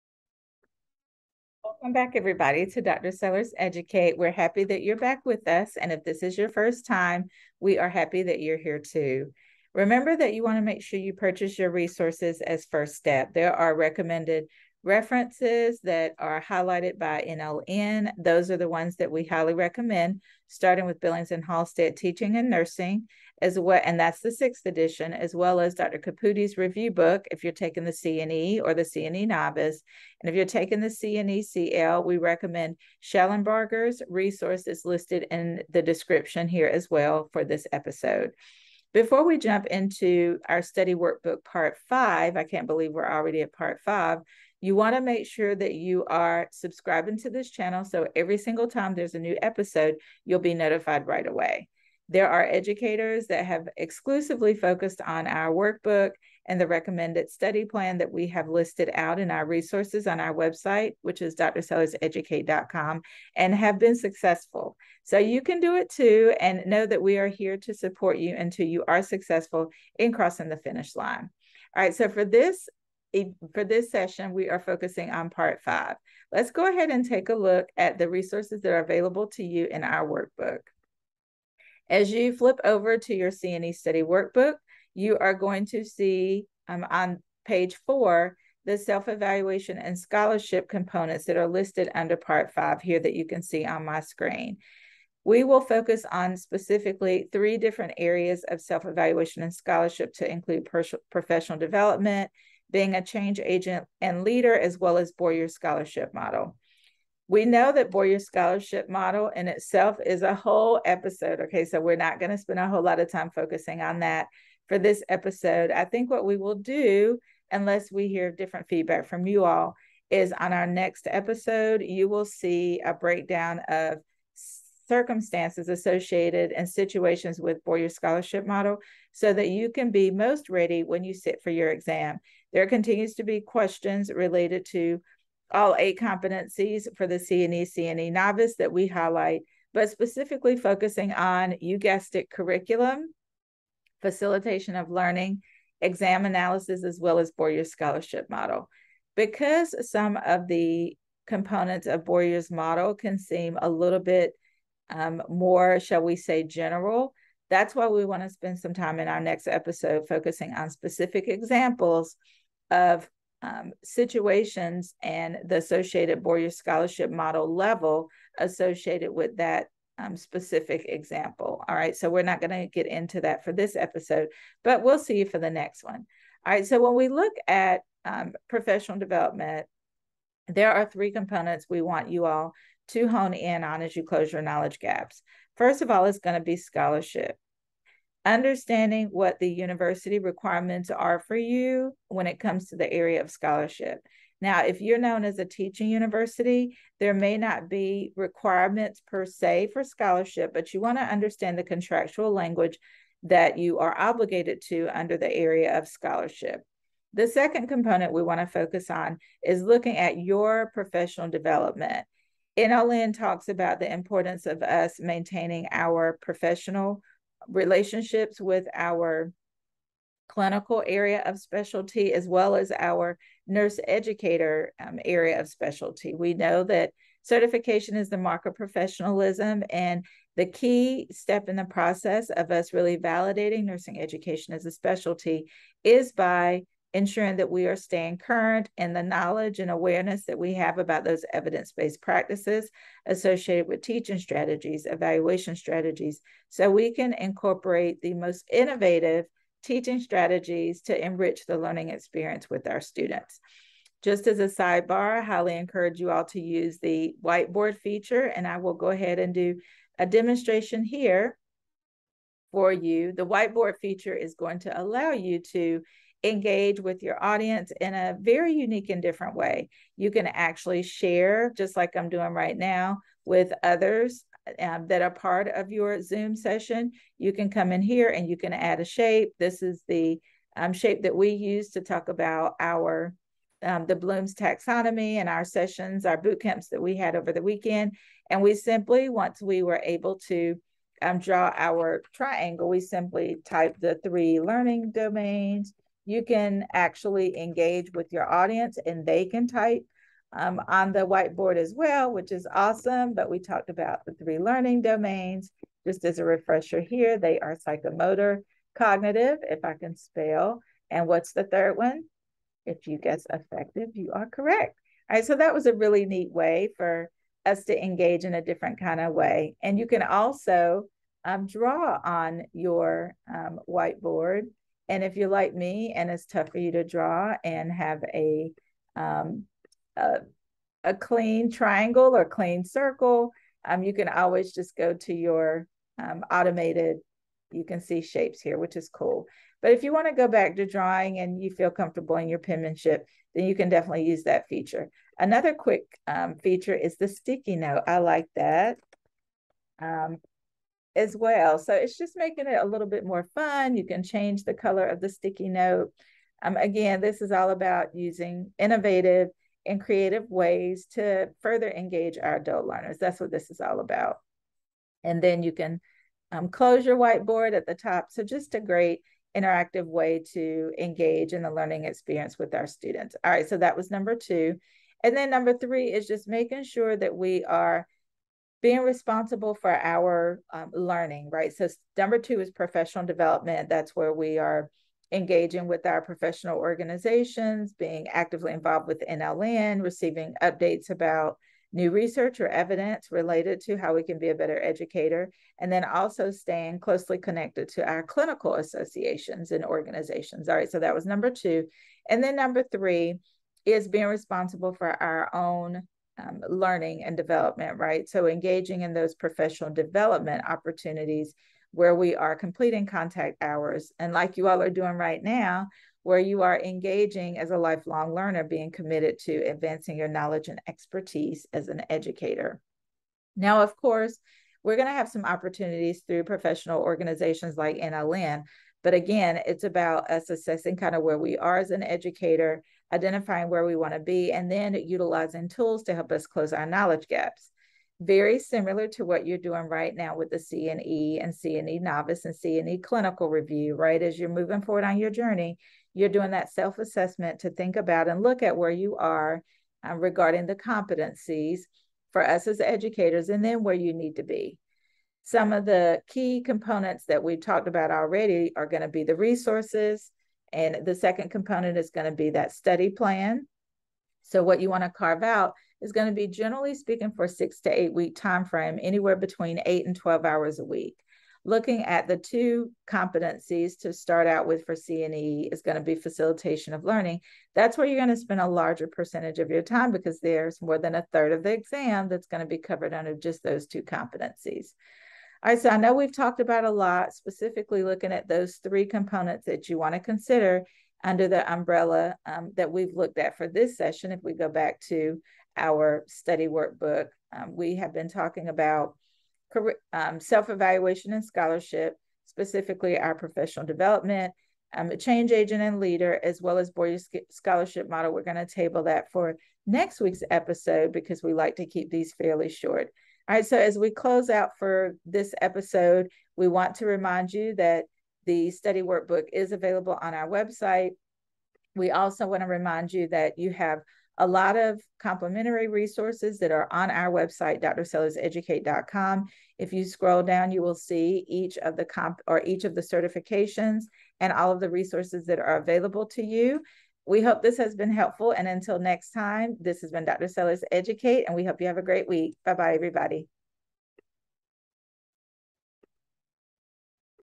Snapshot 69: Study Workbook, Part 5-Professional Development
Nurse Educators~Do you need help passing the NLN CNE® Exam?

What do I focus on related to Professional Development?
Let's get started with Part 5:
3 Focus Areas
💡Scholarship Requirements-What's in the contract?
📌 Professional Organizations-Clinical and Education Specialties-Yes, we need both!
📚Professional Development-Committing to Lifelong Learning

📚Billings, D. M., & Halstead, J. A. (2020). Teaching in nursing: A guide for faculty (6th ed.). St. Louis, MO: Elsevier-Saunders.
📚Shellenbarger: NLN Clinical Nurse Educator Competencies